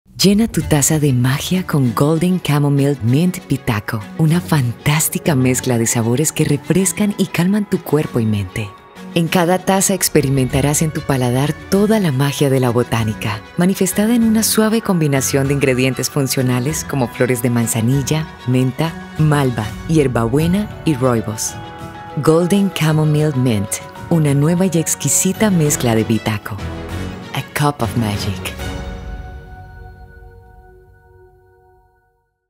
Golden Chamomile Mint, una fantástica mezcla de sabores refrescantes y calmantes.
Golden Chamomile Mint es un té herbal donde experimentarás en cada gota toda la magia de la botánica manifestada en la deliciosa combinación de flores de manzanilla, menta, malva, hierbabuena y rooibos.

Una nueva mezcla de sabores refrescantes y calmantes. Disfruta de sus notas mentoladas, dulces y cítricas, además de su mágico color dorado en cada taza.​